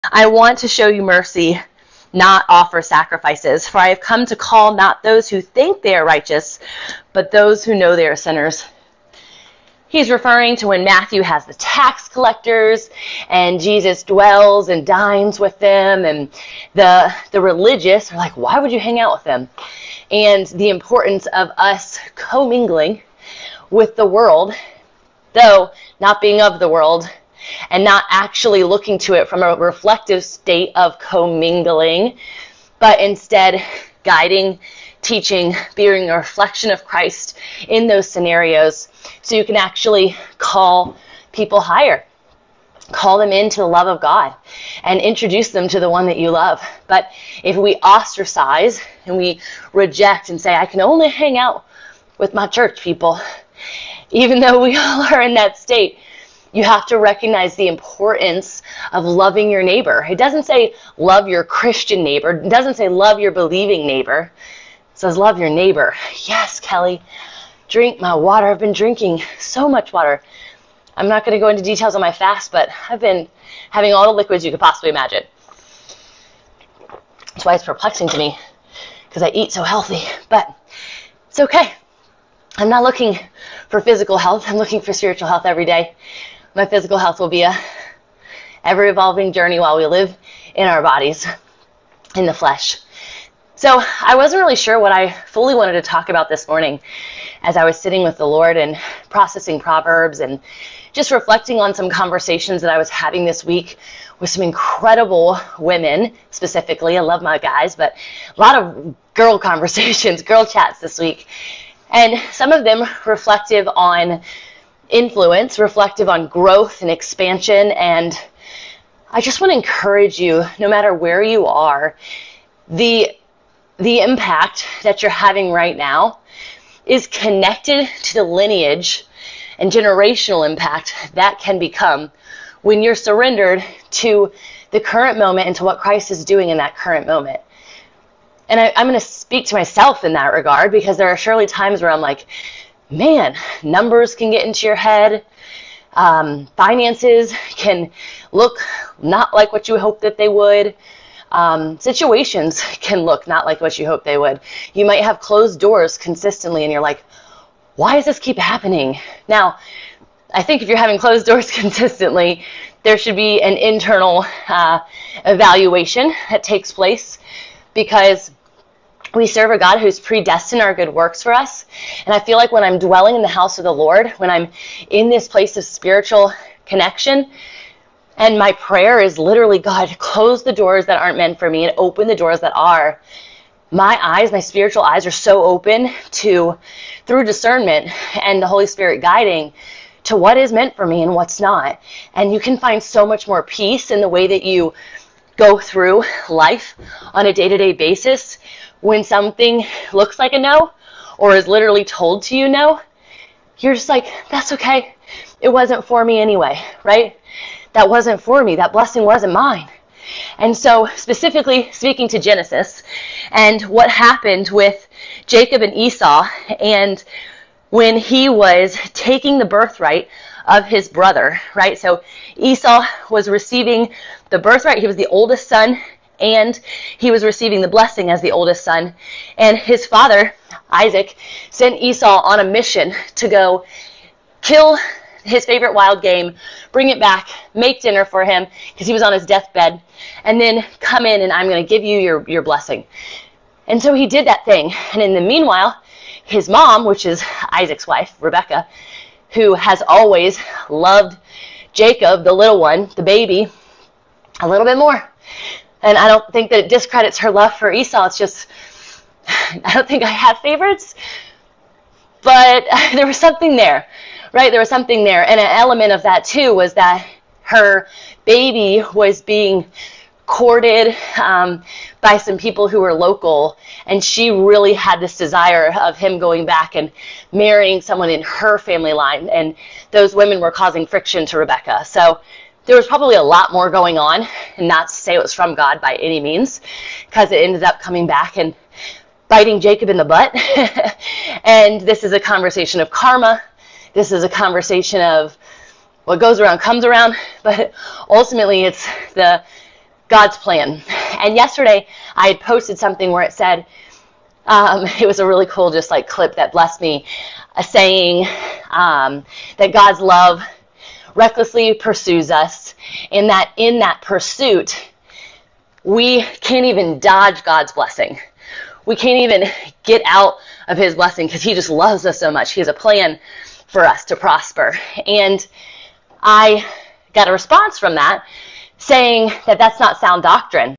The Secret to Happiness: Mercy & Loving Your Neighbor
In this video, we explore the importance of showing mercy and love to all, not just those who we deem righteous. We examine the story of Jesus dining with tax collectors and how we should be co-mingling with the world while also bearing a reflection of Christ. The speaker encourages us to surrender to God's plan and trust that closed doors are not meant for us. She also reflects on the story of Jacob and Esau and the concept of karma. Ultimately, we learn that God's love recklessly pursues us and we are called to love our neighbor and surrender to His plan for us.

To listen to the full podcast episode this was clipped

Business website (Looking for more help in publishing, podcasting and promotion?)